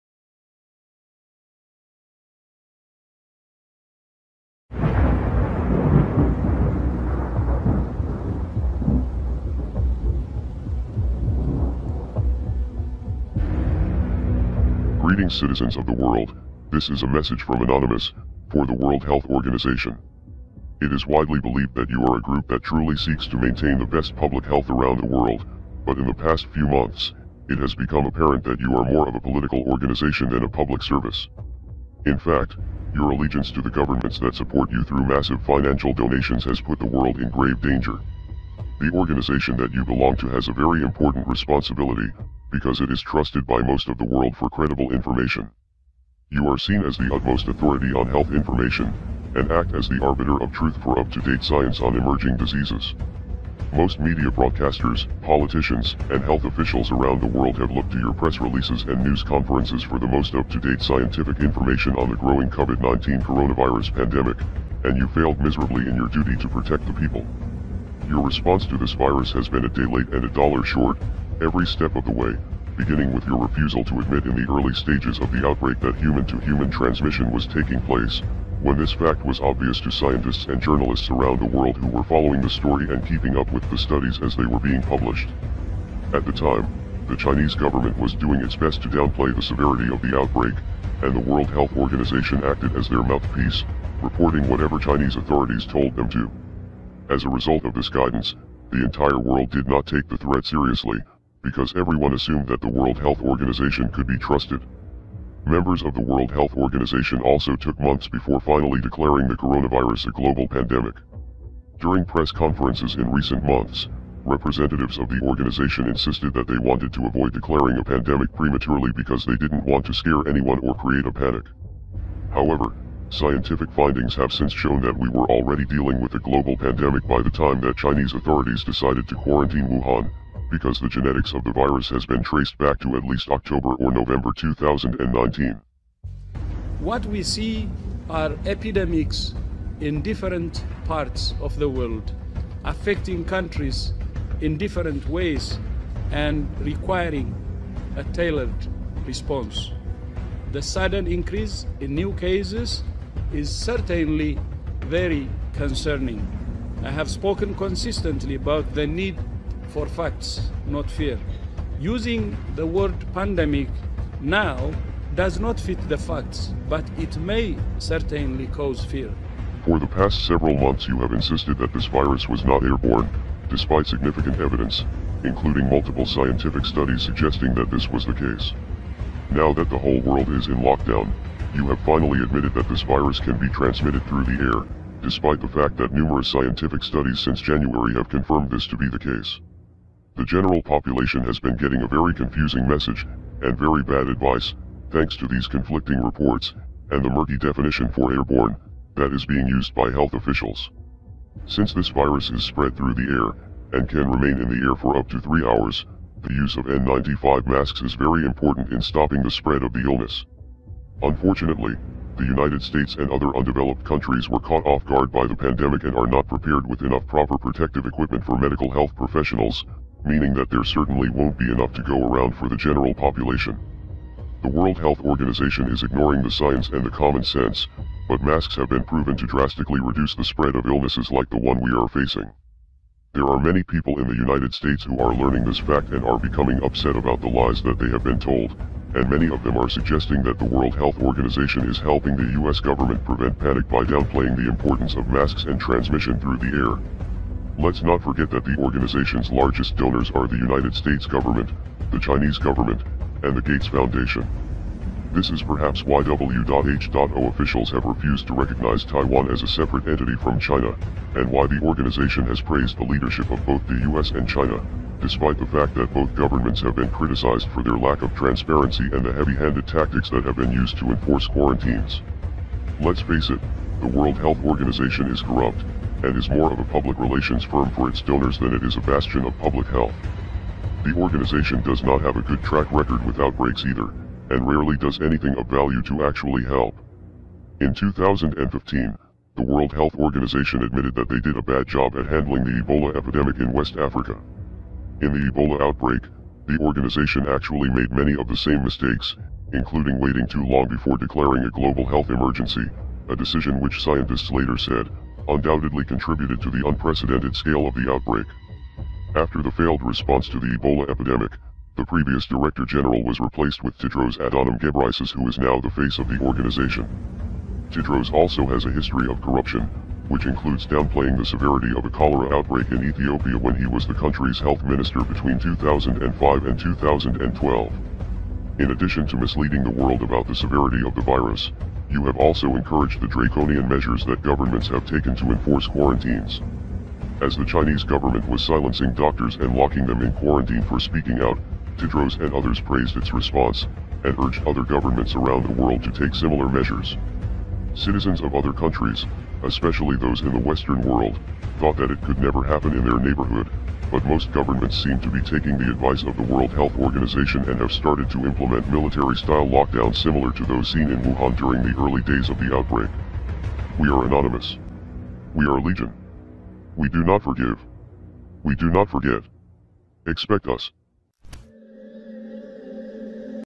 Nuevo mensaje de Anonymous 2020 a la OMS - Corre a verlo antes que lo eliminen (Sub en español)
Anonymous reaparece luego de muchos años inactivo y en este video tira un fuerto msj a la OMS (Organización Mundial de la Salud) toda la información en este video.

Para Donaciones Y Mejoramiento Del Canal Aqui Les Dejo El

♣elChuGamer YT